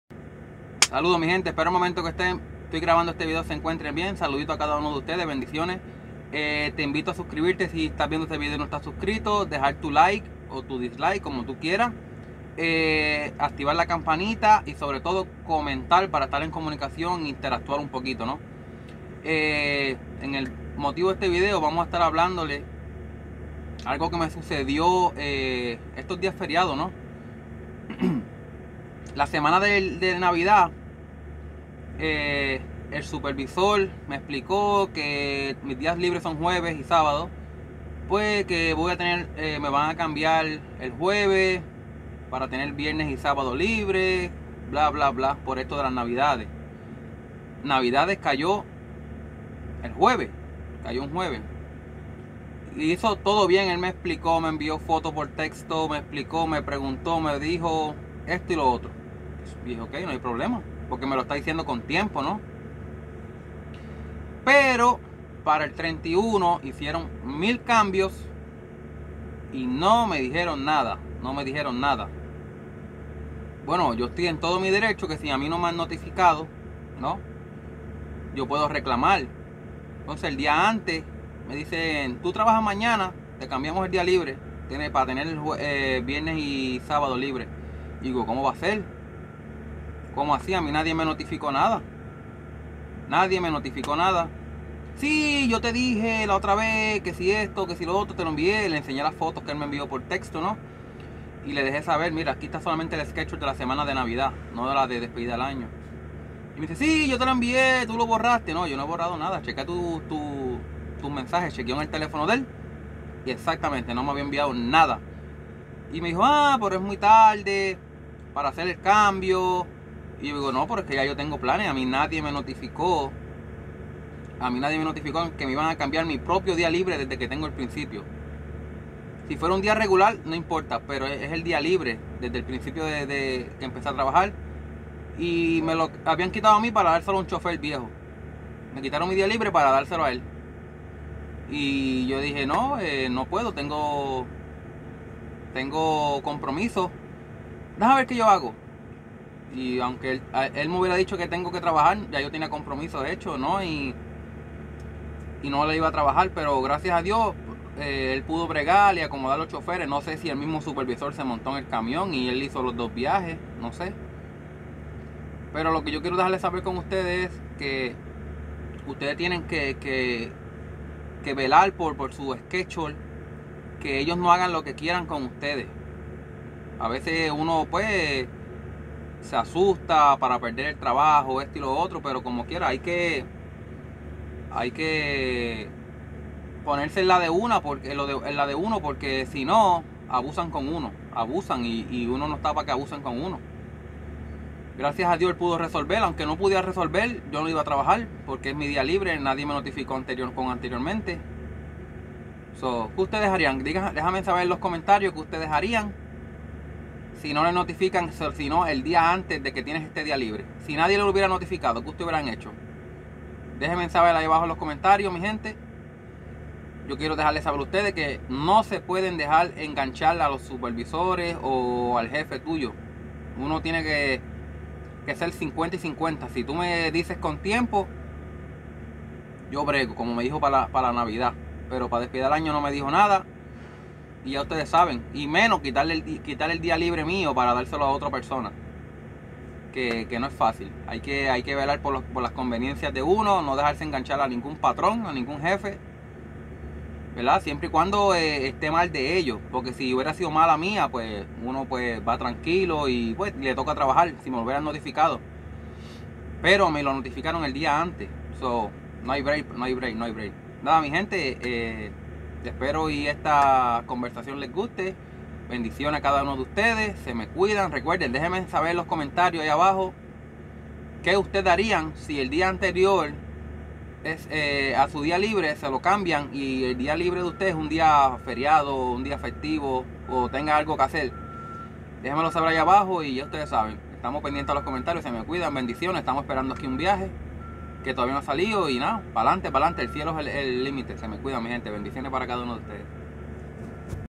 🇵🇷🇵🇷 NO PUEDES DEJAR QUE JUEGUN CON TUS DIAS LIBRES 😡😡
HEKTORIAN CAMIONERO LOCAL SALUDOS MI GENTE EN ESTE VIDEO LES CUENTO QUE ME KERIAN CAMBIAR MI DIA LIBRE PARA DARCELO A OTRO CHOFER NO NO TE PUEDES DEJAR PQ SINO ABUSAN Y HACEN LO QUE KIERAN CONTIGO ✌✌✌